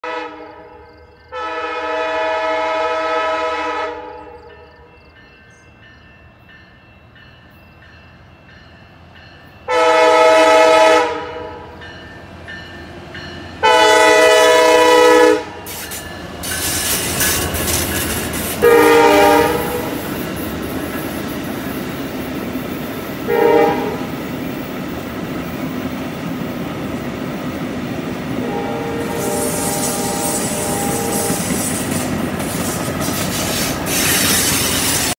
6/9/23 Amtrak 365
6/9/23 09:24 61 degrees at CN MP 202 Charlotte as Amtrak 365 runs slowly over the curve. Full 0:35 video.